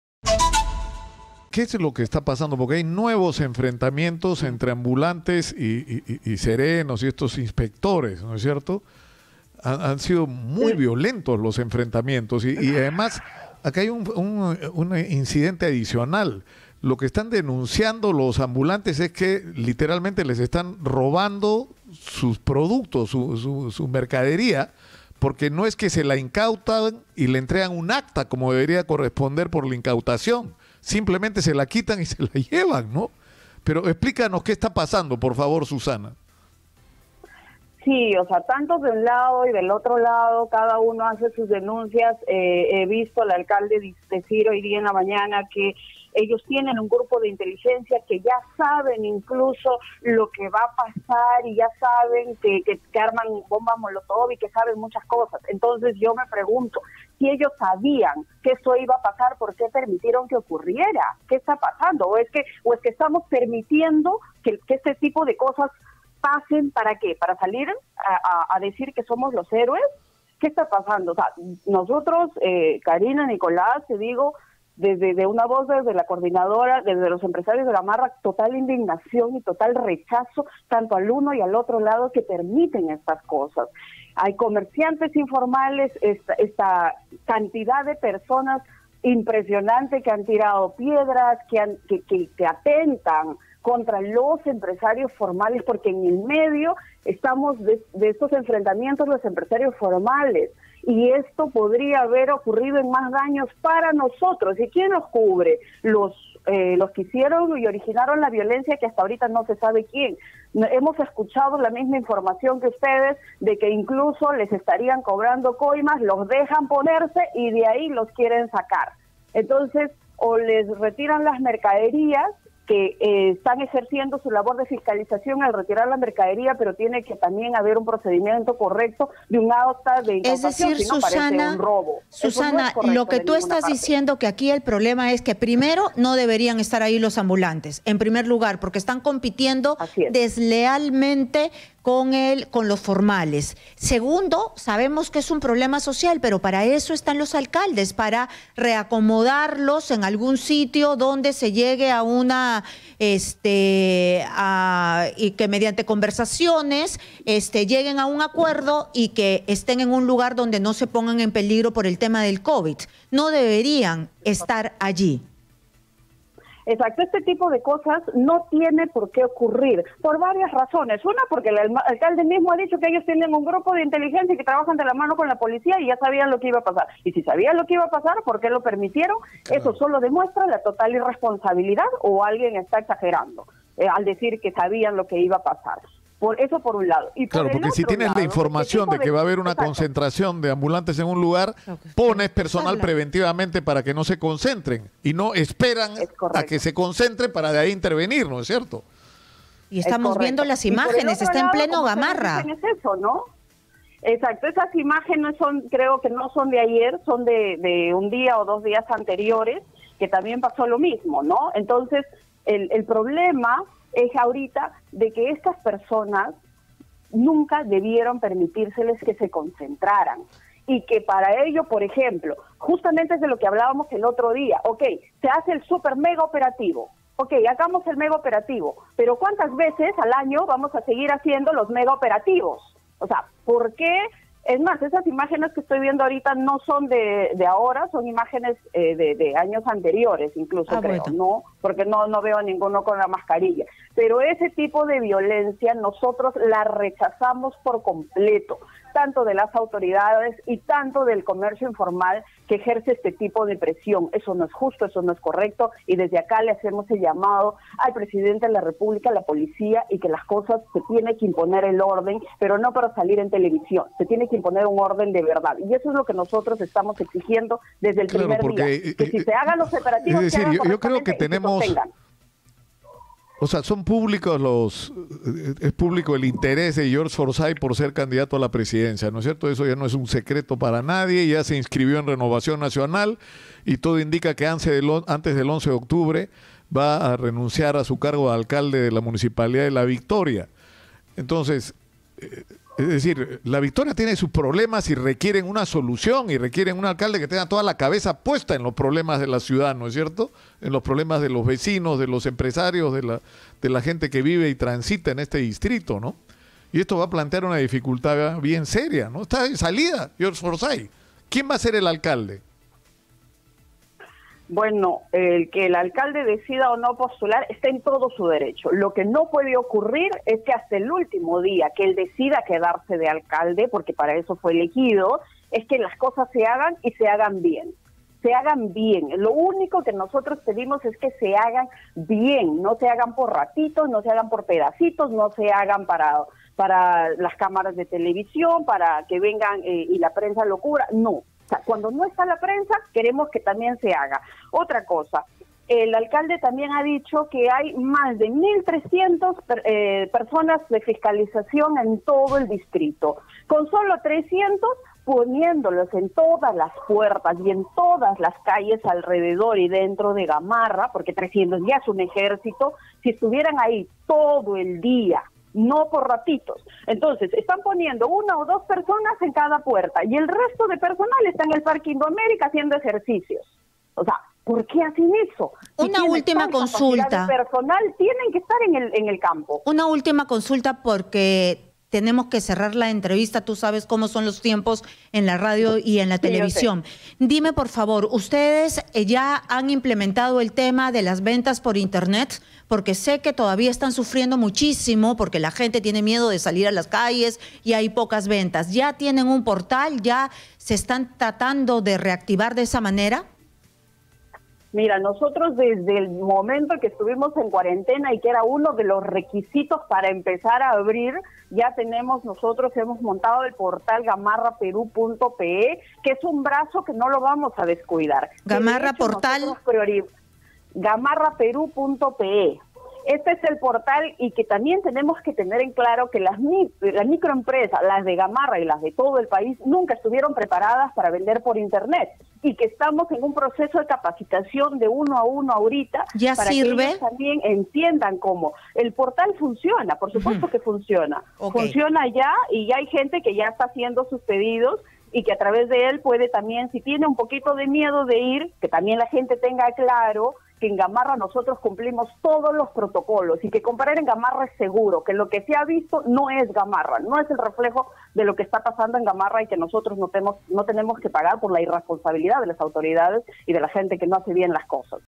🔴🔵Gamarra: Enfrentamientos solo perjudican a los comerciantes formales
La presidenta de la Coordinadora de Empresarios de Gamarra, Susana Saldaña, sostuvo que los enfrentamientos entre ambulante y fiscalizadores, solo ahuyentan a clientes de vendedores formales.

Exitosa Perú
"La voz de los que no tienen voz"

Corporación Universal